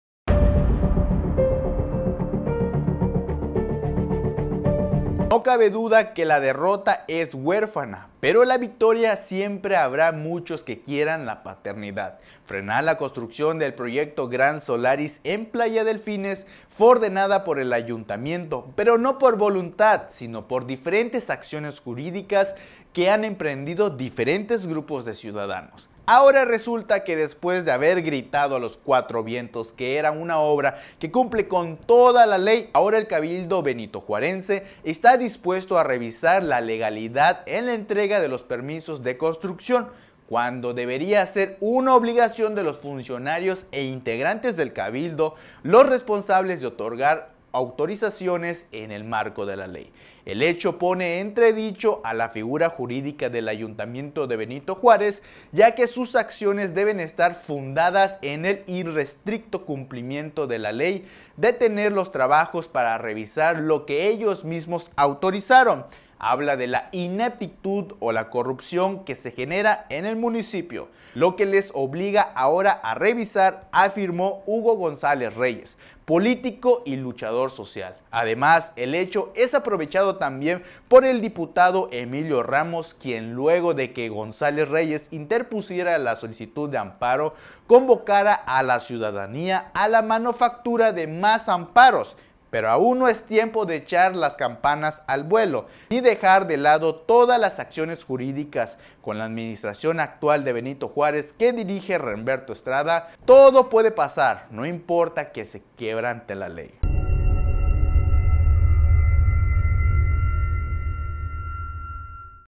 Freno a la edificación en Playa Delfines, gracias a las acciones jurídicas
Entérate de las noticias “EN CORTO”… Freno a la edificación en Playa Delfines, gracias a las acciones jurídicas…